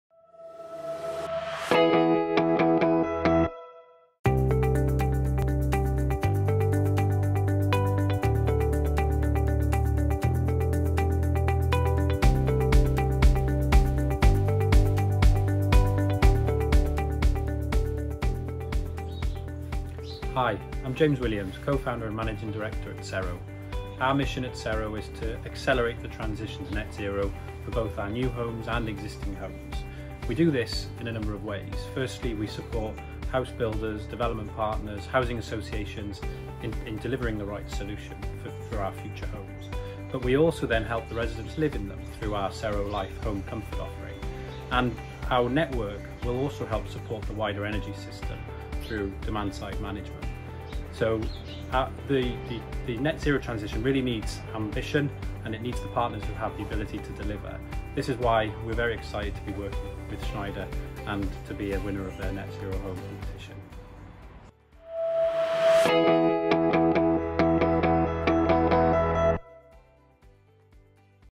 Schneider Electric and Sustainable Ventures Announce the Finalists- Sero
Schneider Electric and Sustainable Ventures have named the four finalists of their Net Zero Home Program that aims to find the most innovative net-zero home solutions to help accelerate the net-zero home market.

Watch finalist James Williams, Managing Director of Sero a company which helps transition net-zero for new and existing homes, discuss how they help homebuilders, housing partners, developers and many more partners by helping them deliver the right solution.

Having each received a £5,000 cash Net Zero Home Grant, the finalists will work with Sustainable Ventures for the next three months to fully explore the potential impact of the technology in creating net-zero homes.

In this video, meet the Managing Director of Sero, winner the Schneider Electric and Sustainable Ventures Net Zero Home Programme.